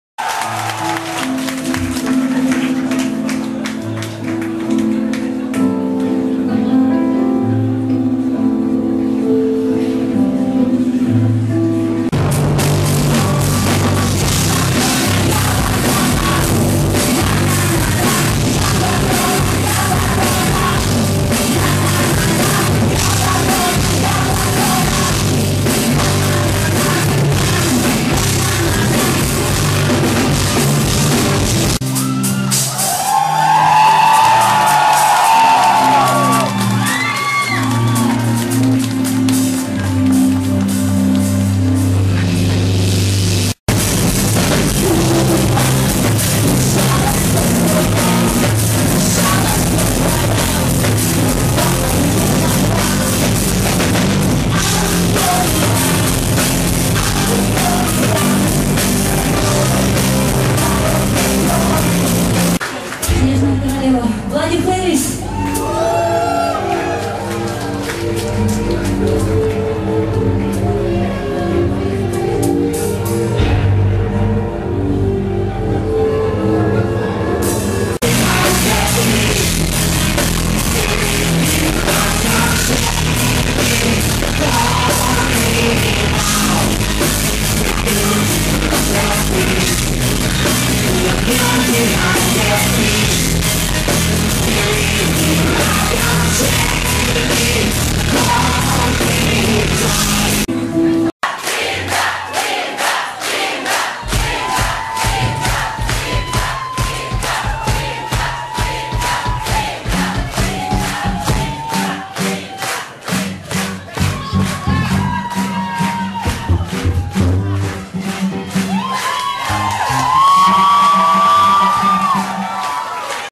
Концерт Линды в Воронеже
16 мая в пабе «Сто ручьёв» состоялся второй за всю историю концерт звезды 90-х Линды в Воронеже. Первое её выступление в нашем городе было 13 лет назад, 5 апреля 1999 года в ДС «Юбилейный». В середине «нулевых» Линда исчезла со сцены, а недавно вернулась, причём теперь она выступает вместе со своим мужем, композитором и продюсером Стефаносом Корколисом, который работал с Деспиной Ванди, Милен Фармер, группой «Металлика» и другими знаменитостями.